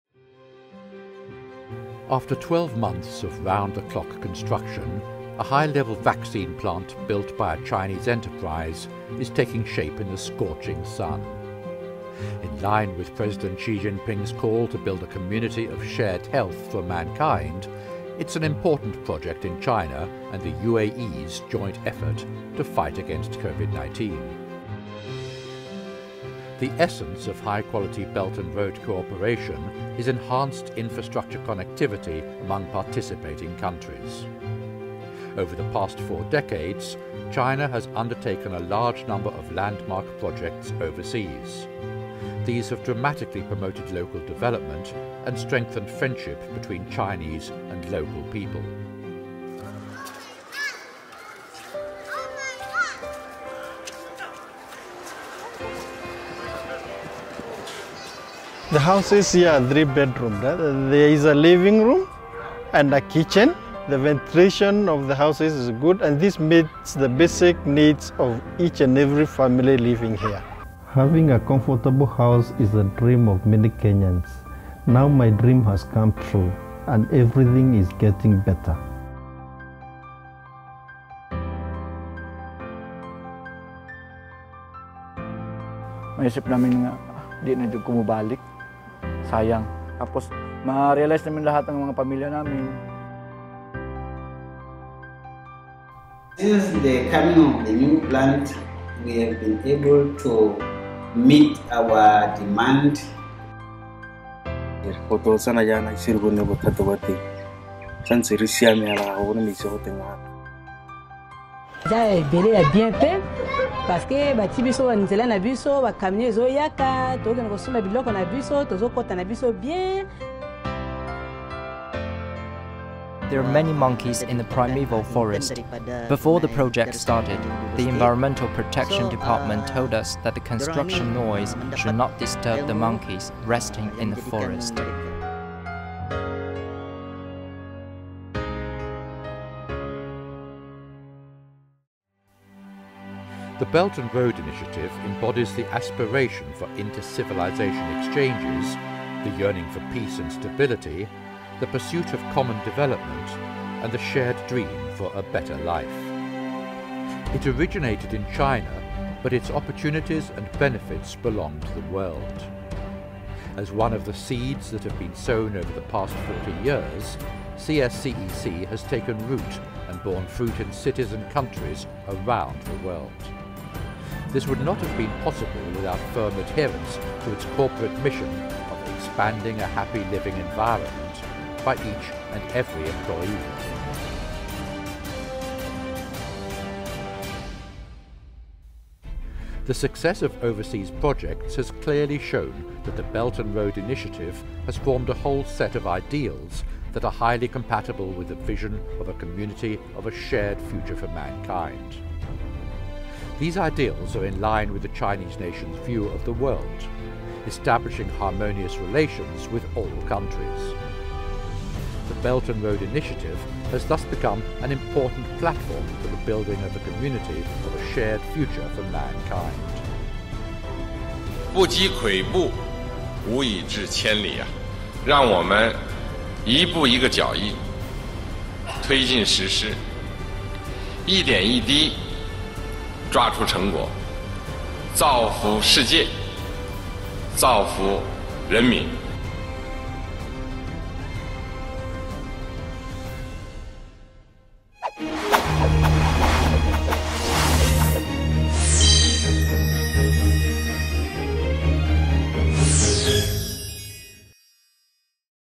Building Lives
Over the past four decades, CSCEC has delivered numerous landmark projects worldwide, boosting local development and fostering global friendships. From building comfortable homes in Kenya to protecting wildlife during construction, CSCEC aims to enhance lives and create a shared future for all.